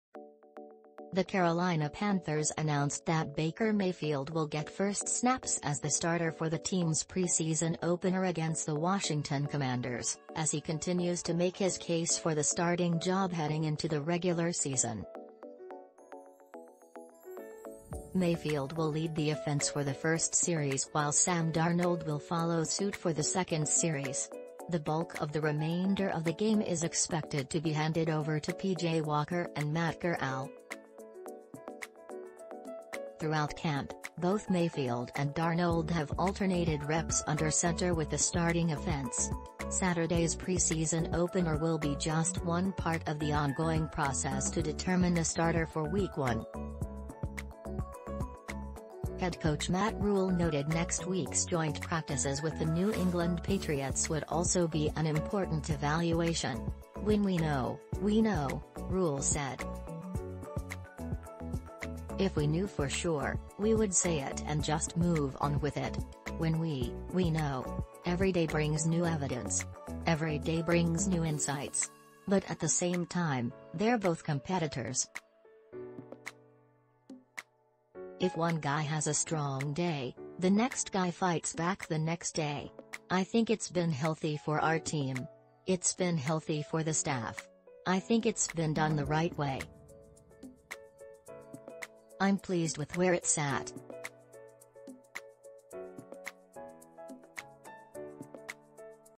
Carolina Panthers announce Baker Mayfield as starting quarterback in
The Carolina Panthers announced that Baker Mayfield will get first snaps as the starter for the team’s preseason opener against the Washington Commanders, as he continues to make his case for the starting job heading into the regular season. Mayfield will lead the offense for the first series while Sam Darnold will follow suit for the second series. The bulk of the remainder of the game is expected to be handed over to PJ Walker and Matt Corral. Throughout camp, both Mayfield and Darnold have alternated reps under center with the starting offense. Saturday’s preseason opener will be just one part of the ongoing process to determine the starter for Week 1. Head coach Matt Rhule noted next week’s joint practices with the New England Patriots would also be an important evaluation.“When we know, we know,” Rhule said. “If we knew for sure, we would say it and just move on with it. When we, we know. Every day brings new evidence. Every day brings new insights. But at the same time, they’re both competitors. If one guy has a strong day, the next guy fights back the next day. I think it’s been healthy for our team. It’s been healthy for the staff. I think it’s been done the right way. I’m pleased with where it’s at.”